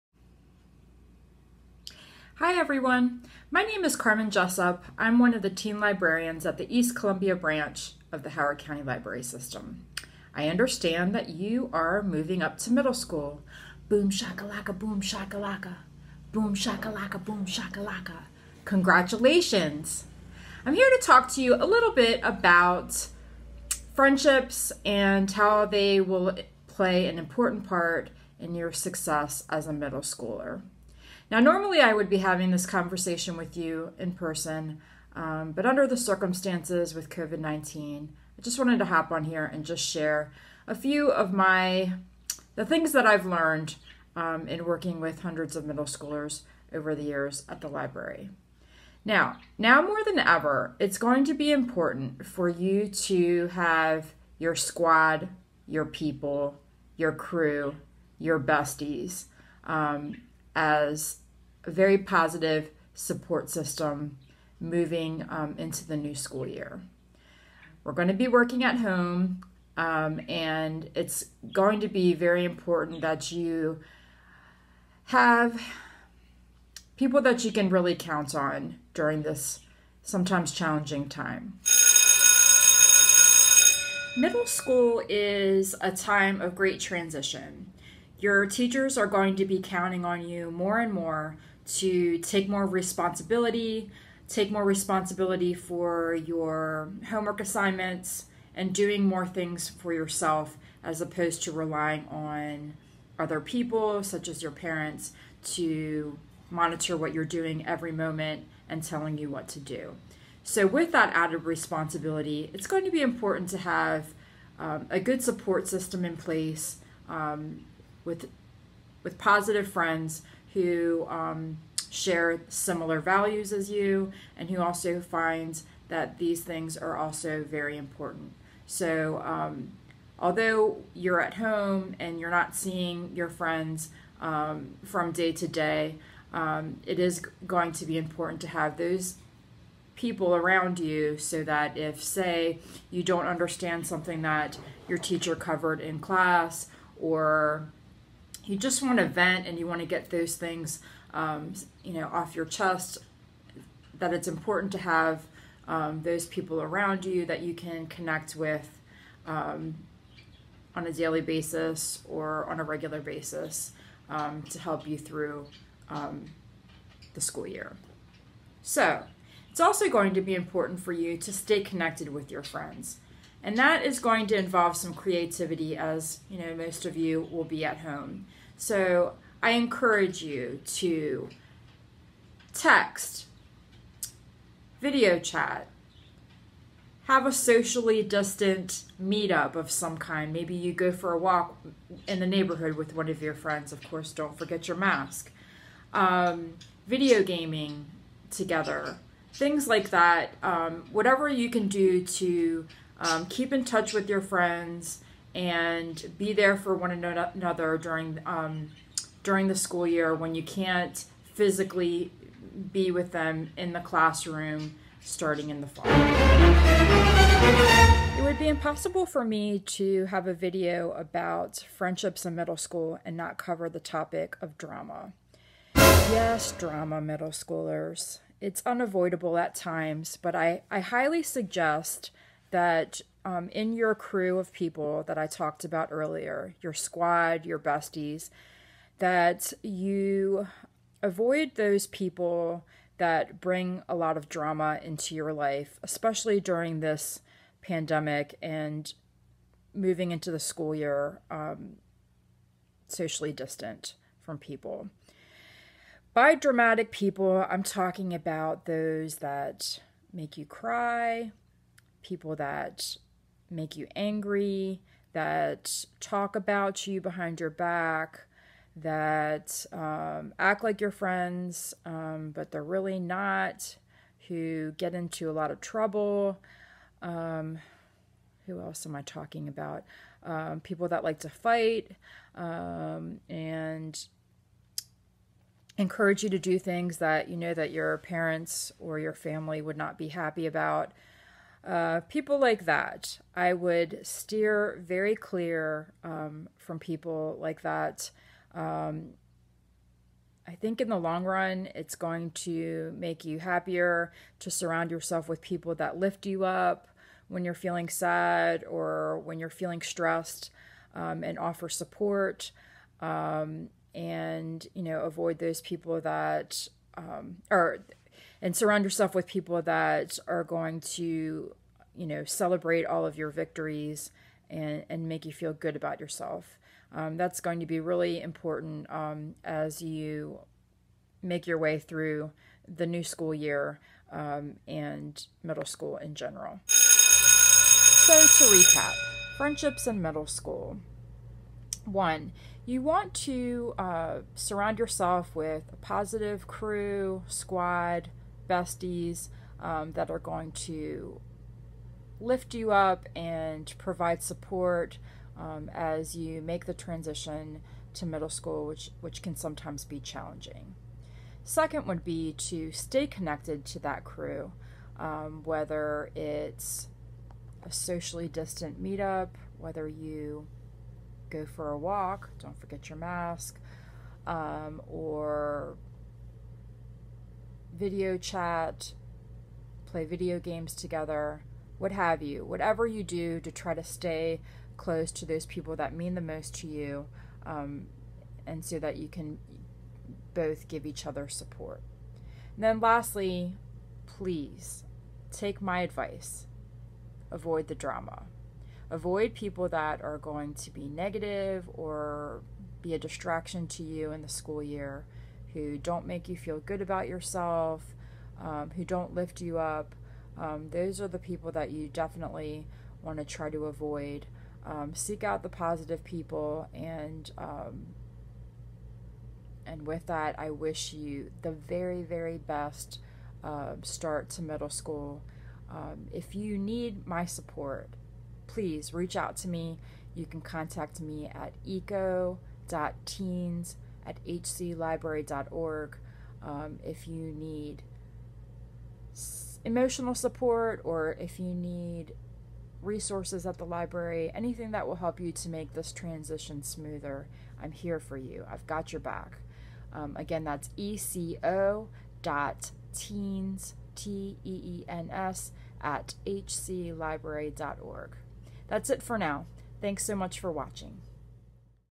Moving Up to Middle School: No Drama
Starting sixth grade?  Learn the secrets to success with our series of videos: handling friendships, managing your time,  staying organized, and using your library card to access homework resources.